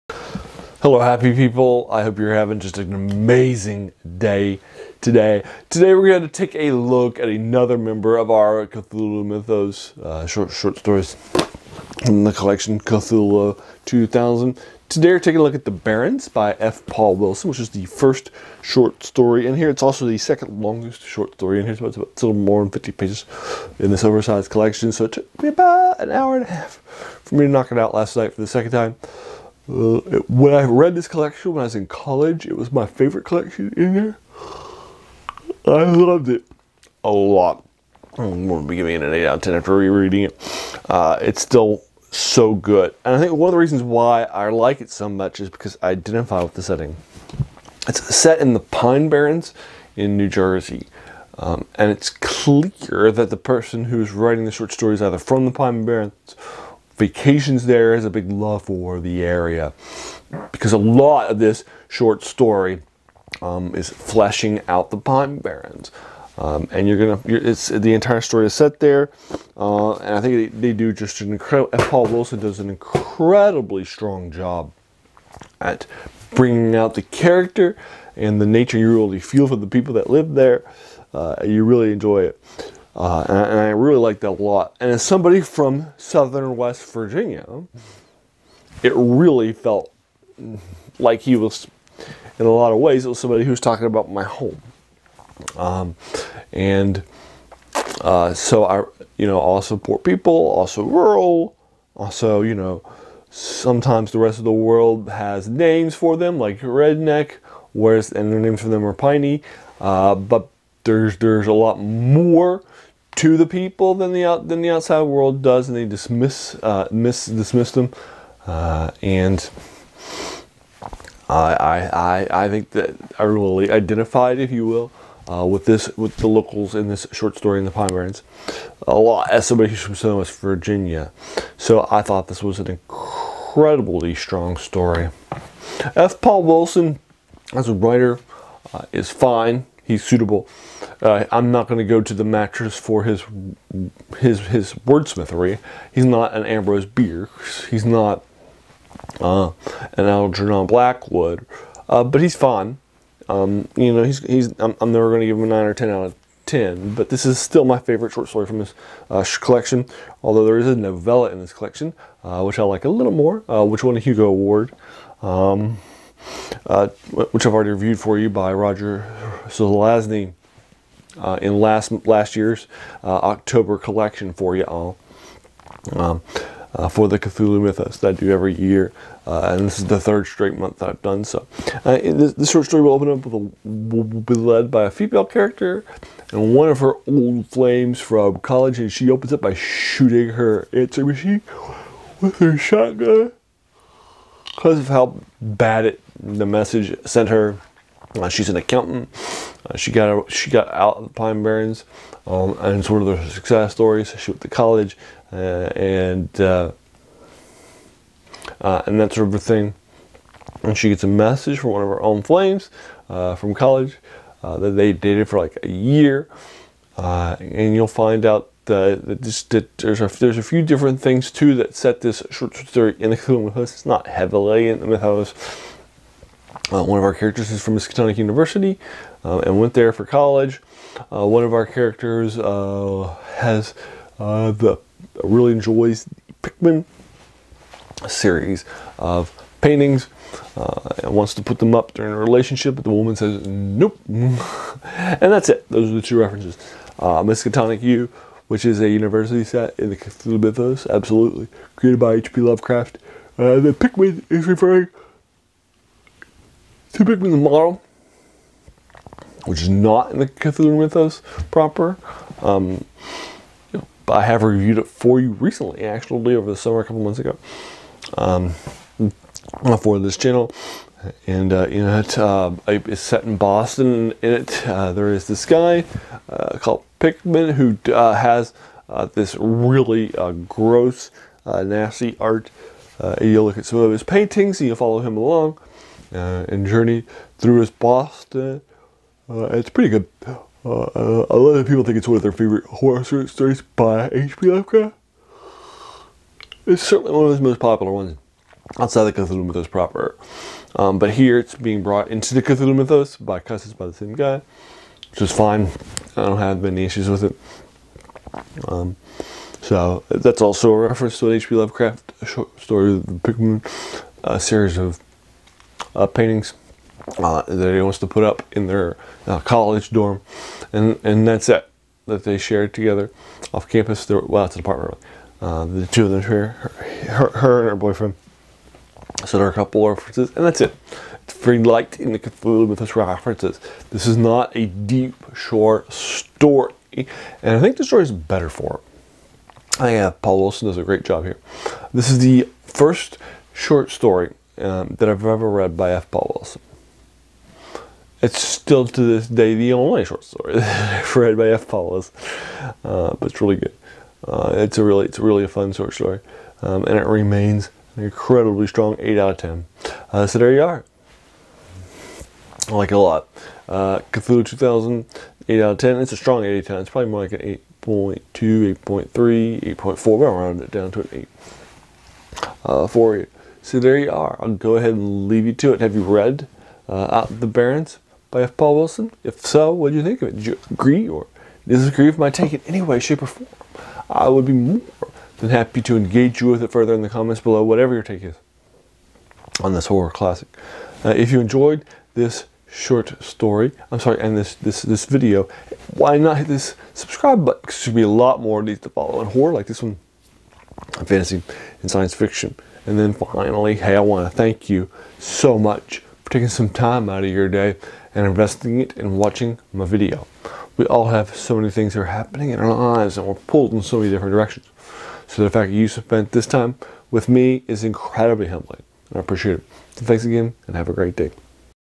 F. Paul Wilson's The Barrens
Hello Youtubers!!!  I hope your day is going happily!

Let’s turn to author, F. Paul Wilson and his Cthulhu Mythos story, “The Barrens.” My copy is in the collection “Cthulhu 2000”. It was my favorite short story in this collection when I read it in college.  What about now? It was first published in 1990 in the collection, “Lovecraft’s Legacy,”ready?

Here is the collection, “Cthulhu 2000”

Have you read The Barrens?  Have you read anything else by F. Paul Wilson? What did you think of it?  Did you agree with my video? Anything in the video or the story that you’d like to discuss?  Sure; let’s chat about it!